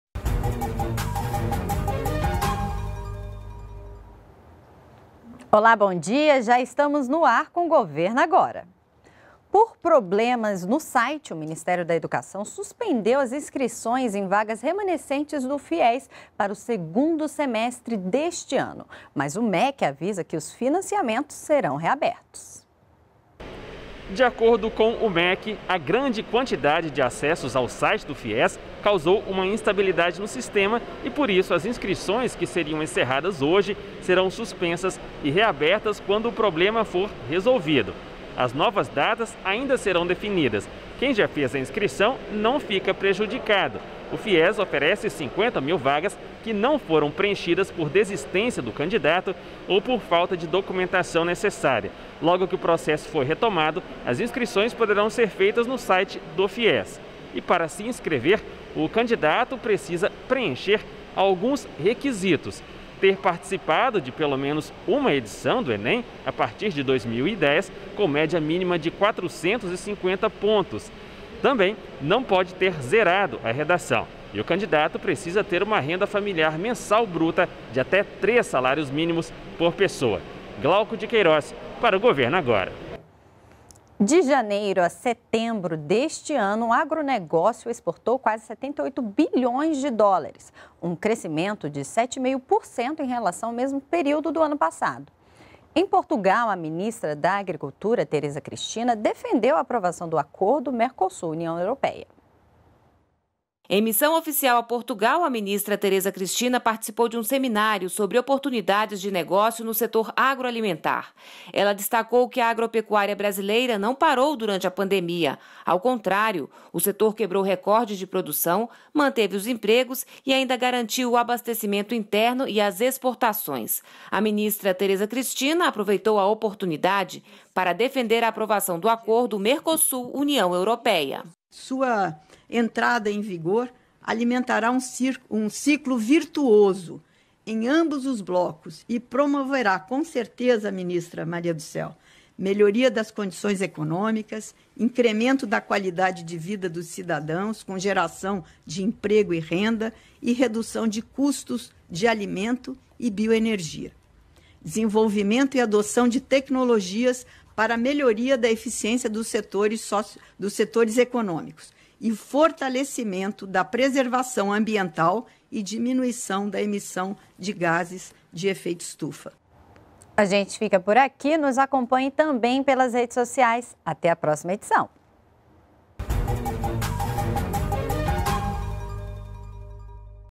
Ministra Tereza Cristina participa de seminário em Lisboa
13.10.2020 - De janeiro e setembro de 2020, as exportações do brasileiras do agronegócio somaram quase US$ 78 bilhões, um crescimento de 7,5% em relação ao mesmo período do ano passado. A ministra da Agricultura, Tereza Cristina, que está em Portugal, falou sobre a importância da agropecuária brasileira e defendeu a aprovação do acordo Mercosul-União Europeia.